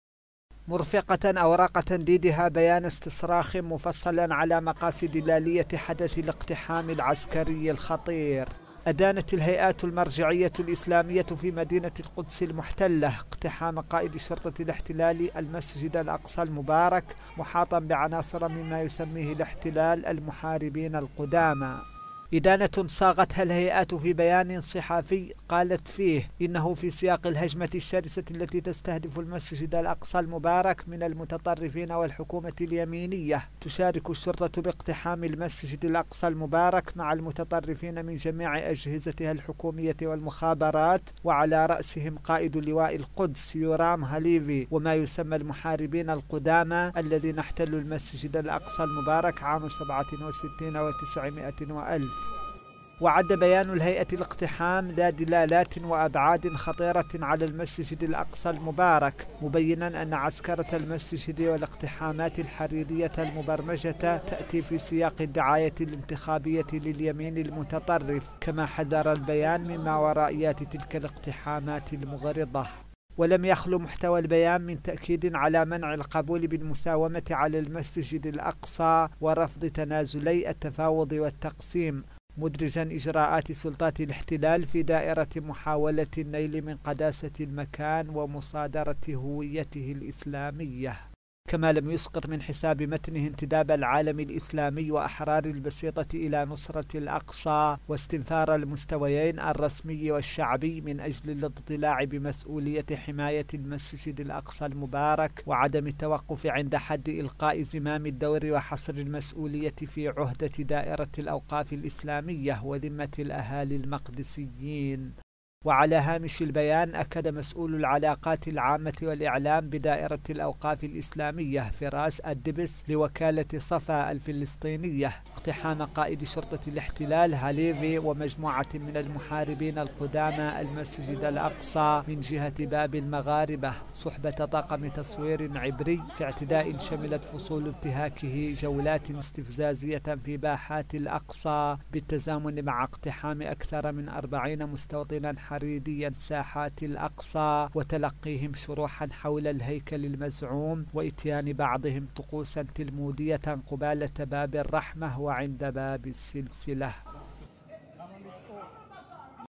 هيئات فلسطينية تندد باقتحام الأقصى من طرف قوات الاحتلال تقرير اشريف ولد محمد يحيى.
نددت هيئات فلسطينية اقتحام عناصر من قوات الاحتلال الإسرائيلية المسجد الأقصى المبارك، مضيفة في بيان مشترك صادر عنها أن الاقتحام له دلالات وأبعاد وصفتها بالخطرة.
وقال البيان إن الاقتحامات الجديدة للأقصى، تأتي في سياق الدعاية الانتخابية لليمين المتطرف، محذرا من ما ورائياتها.
تقرير: اشريف ولد محمد يحيى.